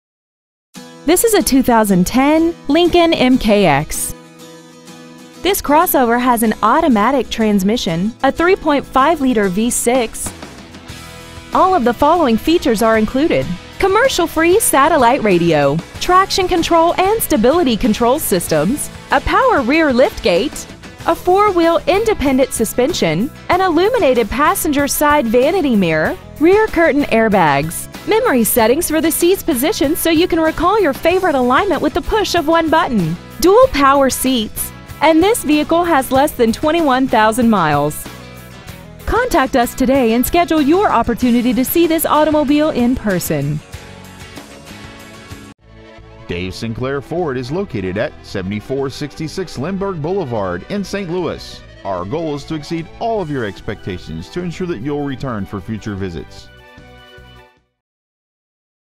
2010 Lincoln MKX St. Louis MO
Year : 2010
 Make : Lincoln
 Model : MKX
 Engine : 3.5L V6
 Trans . : automatic
 Exterior : White Platinum Tri-Coat - (White)
 Miles : 20,646
 Interior : Cashmere Leather
 Stock : P12133400

 7466 Dave Sinclair Memorial Highway

 2010 Lincoln MKX St. Louis MO